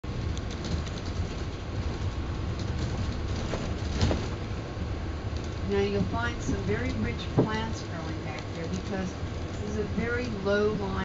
Lower Ninth Ward, 5 Years After Katrina
It took me awhile to realize that this used to be a densely populated neighborhood.  All that was left now was the occasional driveway.  Amazing at how quickly nature moved to reclaim the space.